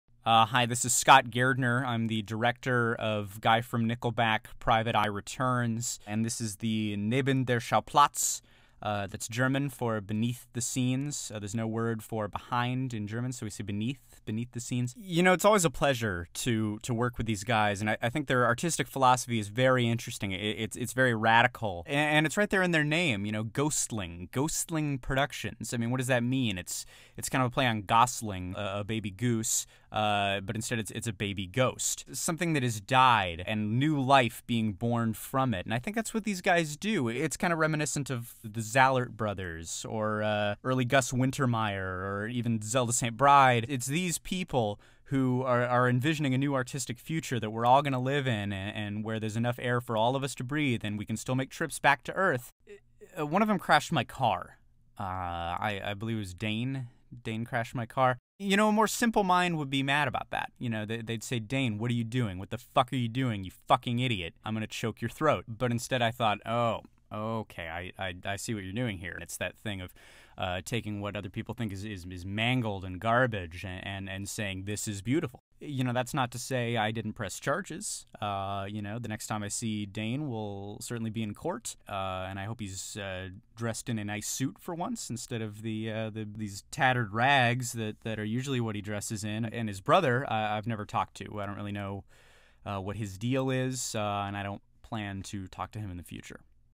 Guy From Nickelback: Private Eye VFX
How we put Guy From Nickelback in the sky using VFX!